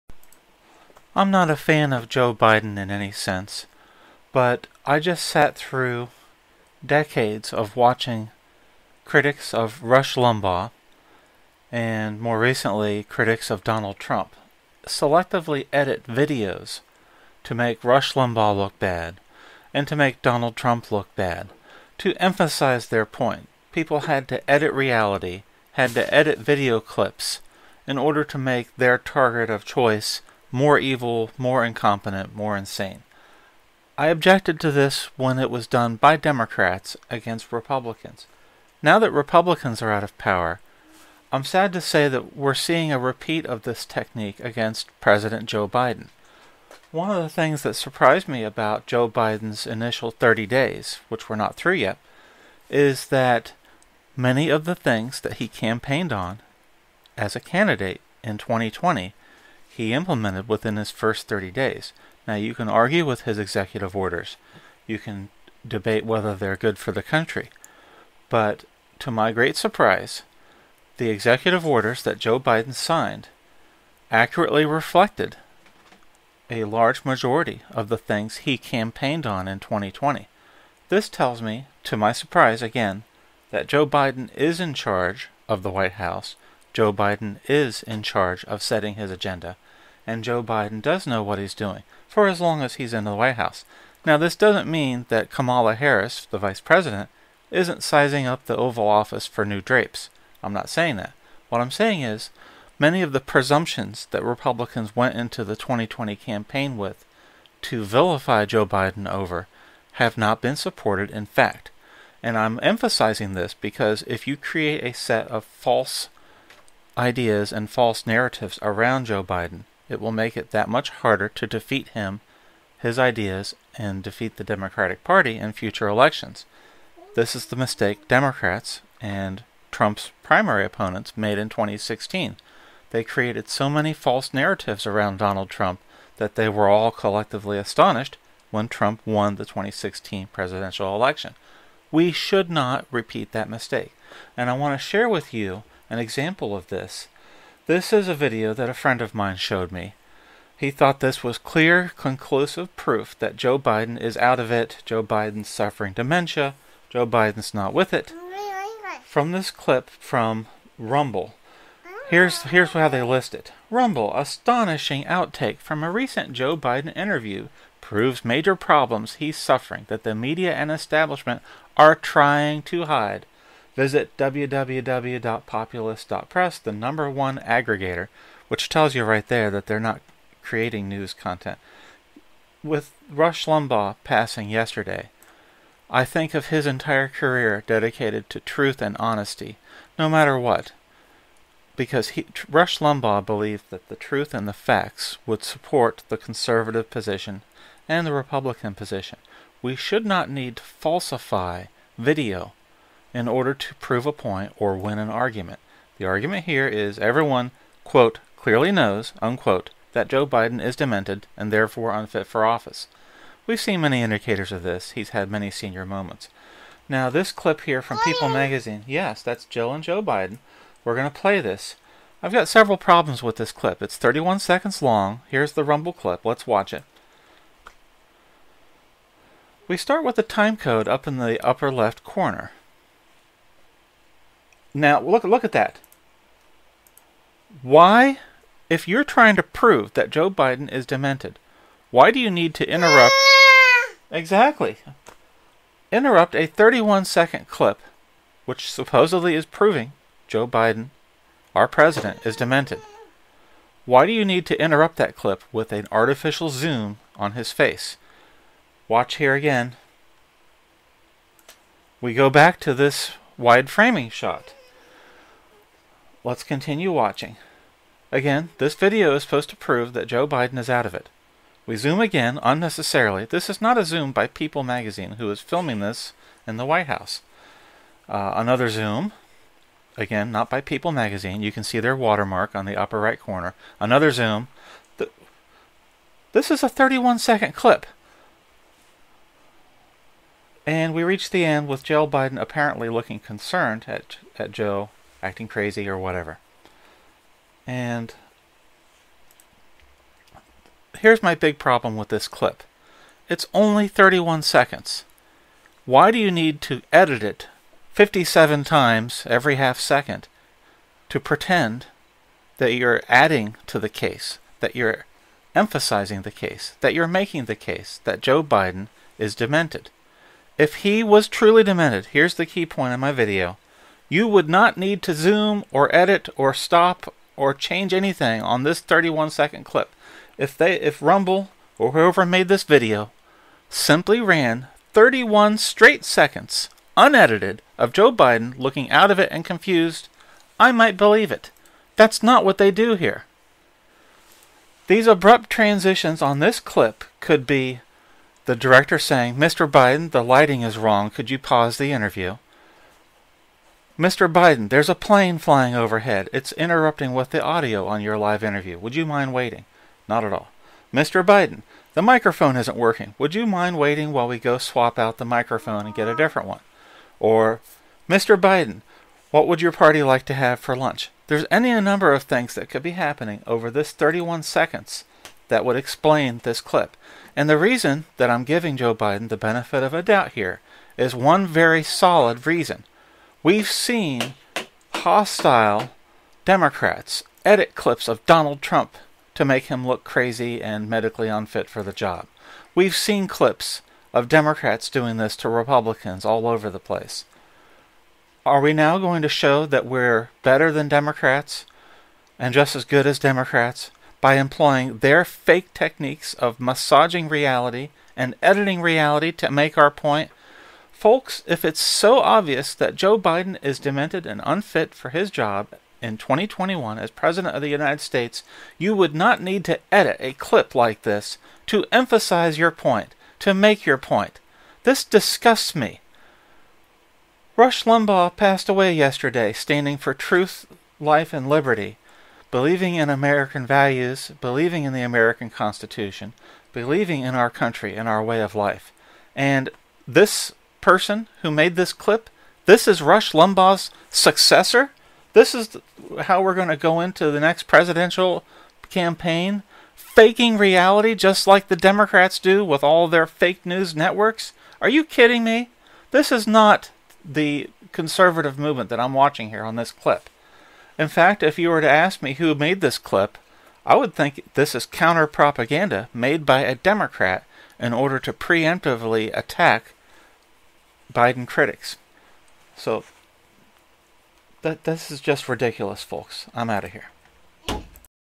This is Not How You Defeat Biden!
It's a given assumption with many Americans that Joe Biden is mentally unfit for the job he's in. But for years we've watched critics of Republicans create video clips that are edited to massage reality to "make their point better", showing crazy Trump or crazy Rush Limbaugh or crazy Mitch McConnell. Now this clip is floating around this week supposedly showing President Joe Biden mentally out of it, with a concerned First Lady looking on. In 31 seconds, the clip has to repeatedly zoom and restart at different times.

Folks, if we have to edit reality to make it look like Biden is worse than he is, we've lost the battle and we're just like those who edited clips of the other side. We should be better than this. Win against President Biden on the issues, on policy priorities, not with edited news clips like this that could be made in an IDENTICAL MANNER to smear any Republican.

If we really need to edit clips to make Joe Biden look bad, then he's not as deranged as some would suppose. This is the successor to Rush Limbaugh's conservative message of truth seeking? If so, our quest to defeat Biden and the Democrats is lost before it even has started.